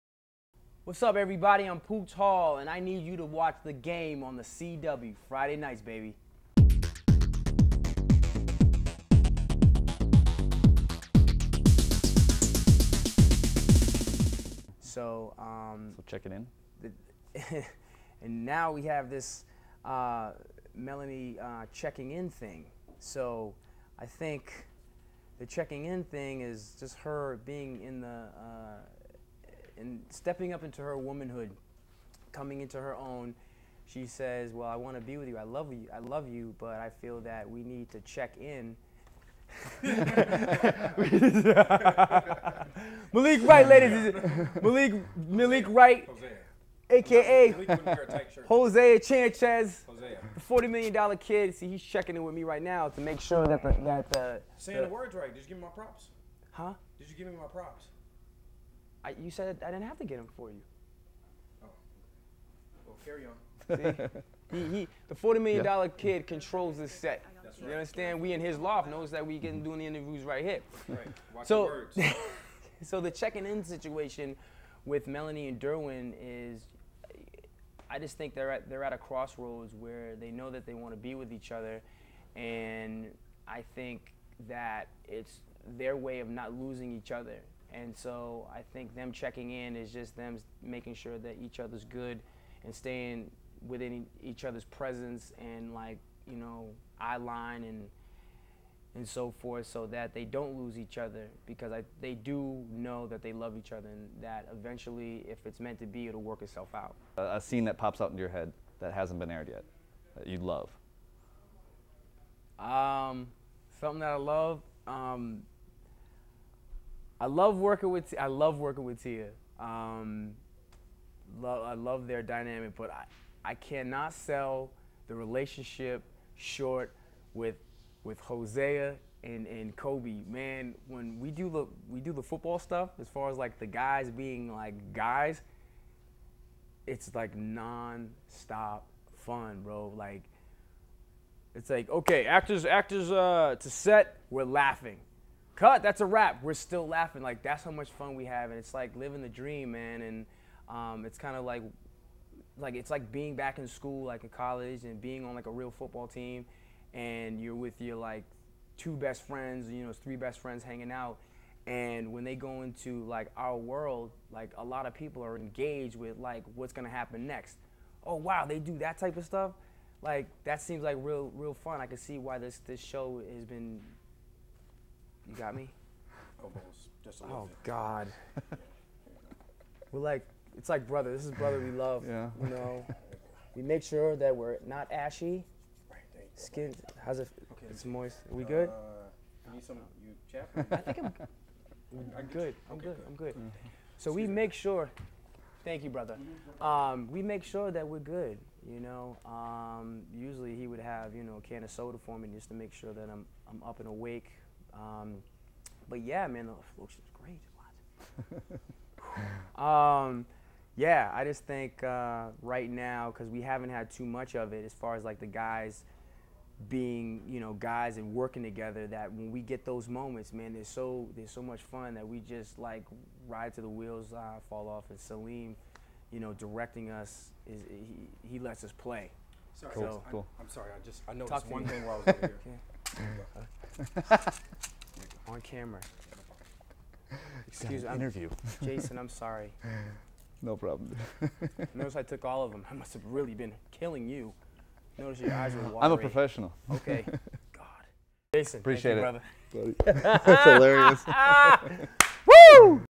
The Game 3: Hosea taking care of Pooch
While we interviewed Pooch Hall about his favorite moments on The Game, Hosea Chanchez made sure Pooch was at his best. Check it out!

Host: Jason C. Sting
Produced by: Catalina Walsh

Property of Tribune Interactive